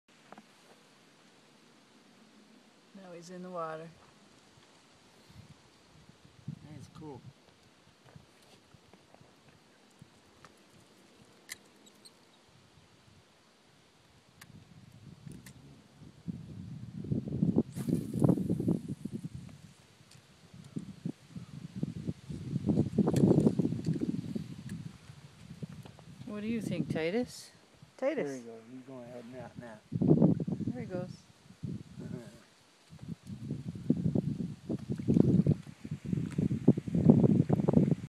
Spiny Soft Shelled Turtle baby 2
A baby Spiny Soft Shelled turtle we found on the Mississippi River.